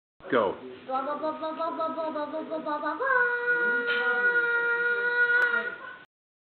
Caitie sings a song of her own composing
Lyrically she's another John Lennon fer sure.  Melodically she's right up there with any of the best rappers.